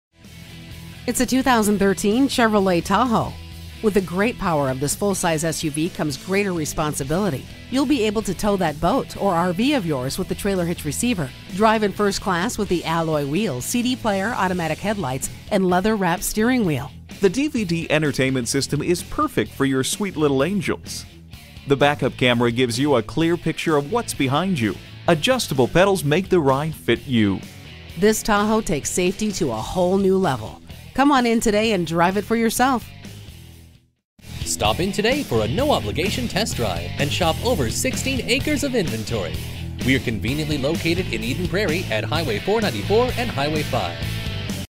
2013 Chevrolet Tahoe Minneapolis St Paul, MN 6160P - SOLD
Year: 2013
Make: Chevrolet
Model: Tahoe
Trim: 4WD 4dr 1500 LT 5.3L V8 DVD
Engine: 5.3 L 8 Cyl. Cyl.
Transmission: 6-Speed A/T
Color: Black
Interior: Light Cashmere/dark Cashmere
Mileage: 16549
Stock #: 6160P
Excellent Condition, GREAT MILES 16,538! LT trim. Third Row Seat, Moonroof, Heated Leather Seats, DVD Entertainment System, Head Airbag, Trailer Hitch, 4x4, Rear Air, Aluminum Wheels, ENGINE, VORTEC 5.3L V8 SFI FLEXFUEL. SEE MORE! KEY FEATURES INCLUDE: Third Row Seat, 4x4, Rear Air, Running Boards, Premium Sound System. Rear Seat Audio Controls, Keyless Entry, Privacy Glass, Steering Wheel Controls, Rear Parking Aid. OPTION PACKAGES: LUXURY PACKAGE includes (DD8) inside rearview auto-dimming mirror, (AN3) front bucket seats with leather-appointed seating, heated front seats, (KA6) heated second row seats, (ARS) second row, power seat release, (DL3) outside heated power-adjustable, power-folding mirrors with memory function and curb-tilt, (E61) rear power-operated liftgate and (UG1) universal home remote, ENTERTAINMENT SYSTEM, REAR SEAT DVD PLAYER with remote control, overhead display, 2 sets of 2-channel wireless infrared headphones and auxiliary audio/video input jacks, SUNROOF, POWER, TILT-SLIDING with express-open and close and wind deflector, AUDIO SYSTEM, AM/FM STEREO WITH MP3 COMPATIBLE CD/DVD PLAYER seek-and-scan, digital clock, auto-tone control, Radio Data System (RDS), speed-compensated volume, TheftLock and USB port, LT PREFERRED EQUIPMENT GROUP Includes Standard Equipment, ENGINE, VORTEC 5.3L V8 SFI FLEXFUEL with Active Fuel Management, capable of running on unleaded or up to 85% ethanol (with gas - 320 hp [238.6 kW] @ 5400 rpm, 335 lb-ft of torque [452.2 N-m] @ 4000 rpm, with E85 ethanol - 326 hp [243.1 kW] @ 5300 rpm, 348 lb-ft of torque [469.8 N-m]

Address:
12475 Plaza Drive